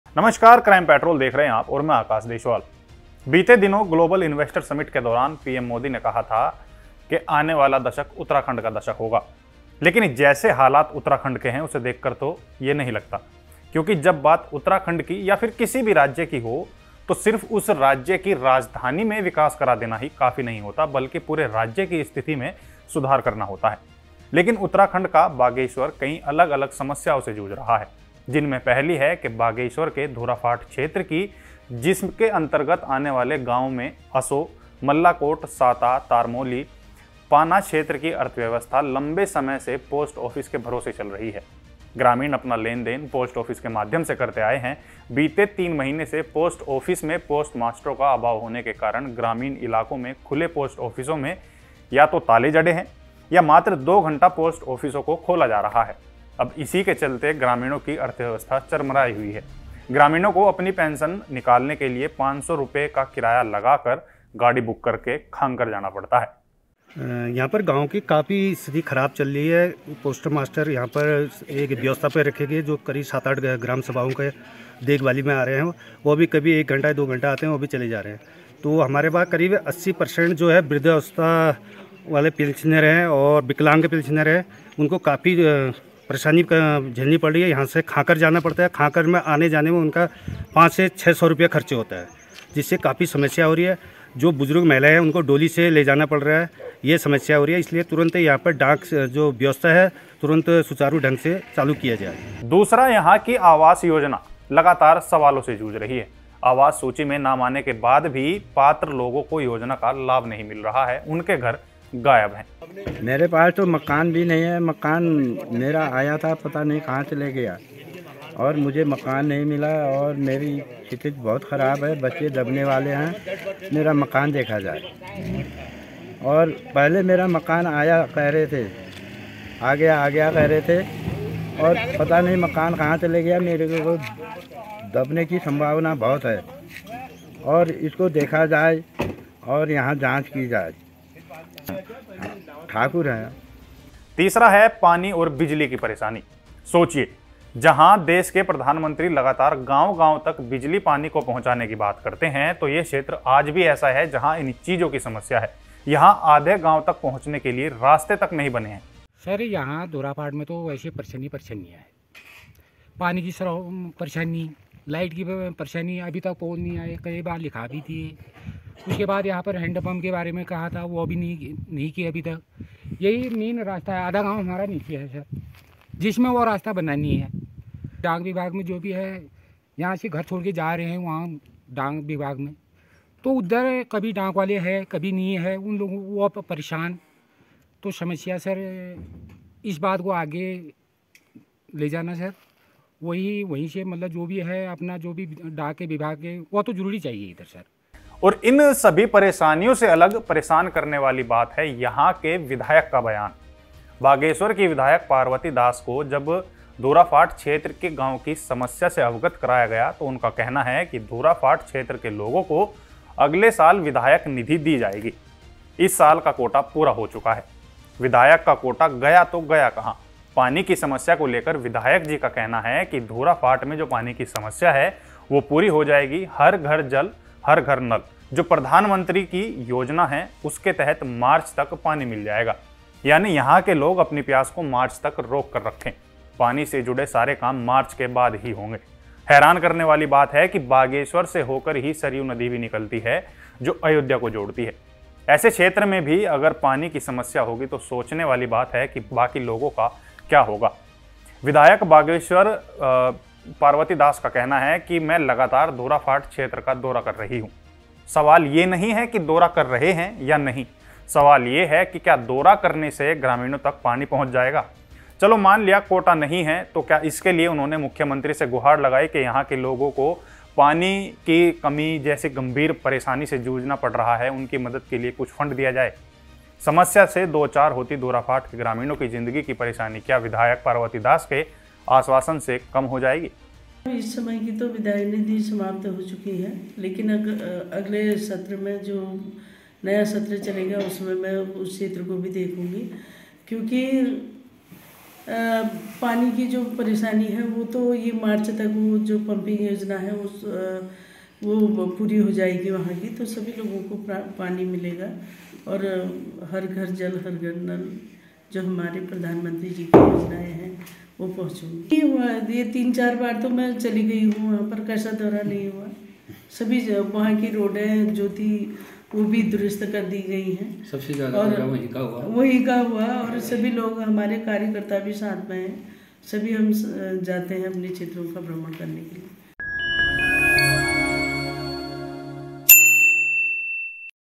प्यास रोक कर रखें, मार्च तक ही नल से मिलेगा पानी
बीते दिनों ग्लोबल इन्वेस्टर समिट के दौरान पीएम मोदी ने अपने संबोधन में कहा था कि आने वाला दशक उत्तराखंड का दशक होगा. लेकिन जैसे हालात उत्तराखंड है उसे देखकर तो ये नहीं लगता. क्योंकि जब बात उत्तराखंड की या फिर किसी भी राज्य की हो तो सिर्फ उस राज्य की राजधानी में विकास करा देना ही काफी नहीं होता बल्कि पूरे राज्य की स्थिती में सुधार करना होता है. लेकिन उत्तराखंड का बागेश्वर कई अलग अलग समस्याओं से जूझ रहा है. जिनमें पहली है बागेश्वर के धूराफाट क्षेत्र की जिसके अन्तर्गत आने वाले गांवों में असों, मल्लाकोट, साता, तरमोली, पाना क्षेत्र कि अर्थव्यवस्था लंबे समय से पोस्ट आफिस के भरोसे चल रही है। ग्रामीण अपना लेन देन पोस्ट आफिस के माध्यम से करते आये है,